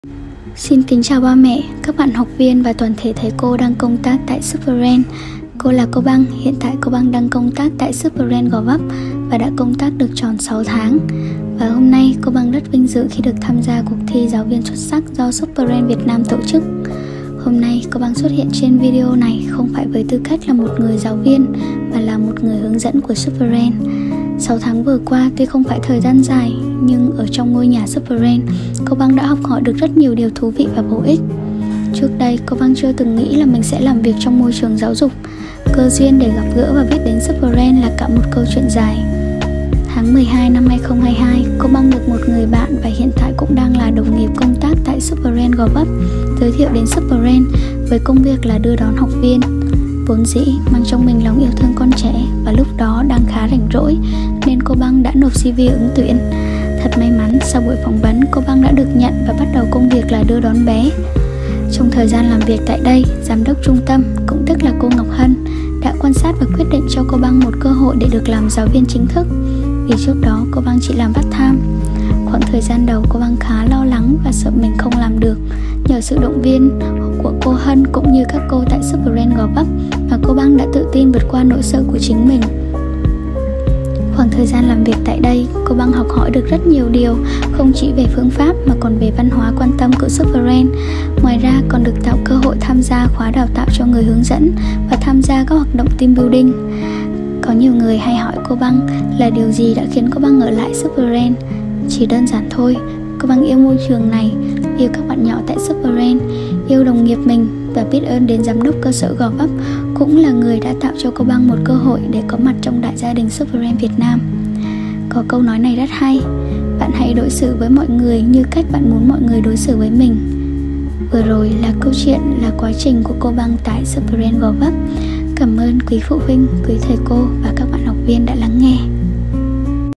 Người hướng dẫn: Nguyễn Khánh Băng - Superbrain Gò Vấp
CUỘC THI GIÁO VIÊN XUẤT SẮC NĂM 2023
Người hướng dẫn: NGUYỄN KHÁNH BĂNG
Cơ sở: SUPERBRAIN GÒ VẤP

💕 Hãy đối xử với mọi người bằng cách bạn  muốn mọi người đối xử với mình 🫶🏻
 Cô Băng cảm ơn quý phụ huynh, quý thầy/cô và các bạn học viên đã lắng nghe câu chuyện của cô Băng tại Superbrain Gò Vấp ♥️